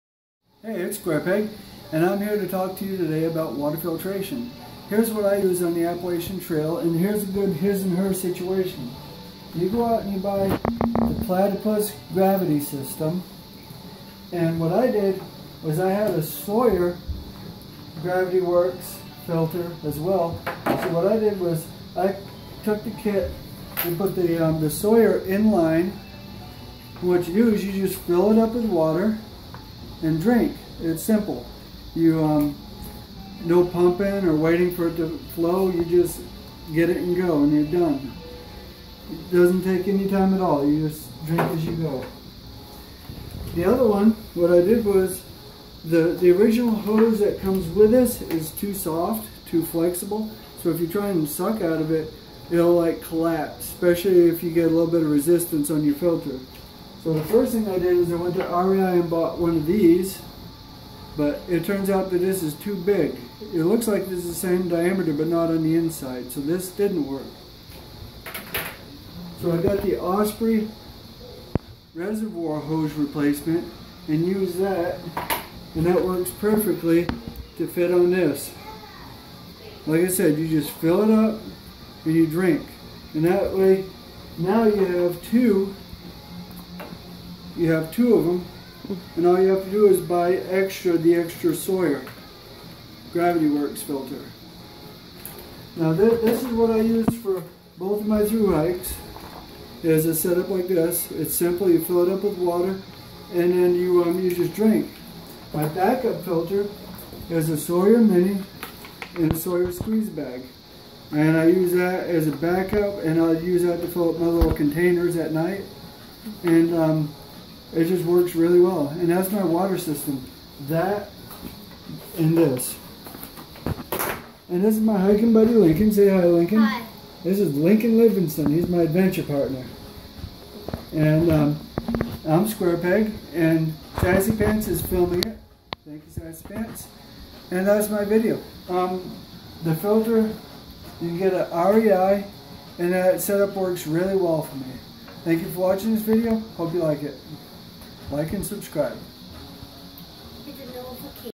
Squarepeg's water filtration system.
Here's my water filtration system for my next thru-hike which is basically the same system that I've used on my thru-hikes in 2015 and 2016.

I like placing my water filters in line on my Platypus hose. This makes water filtration a breeze as you just fill the Platypus with unfiltered water and filter as you drink. This is a simple and lightweight method of filtering water. I use a Sawyer Mini with a squeeze bag as my backup water filter and as a small filter system for slackpacking.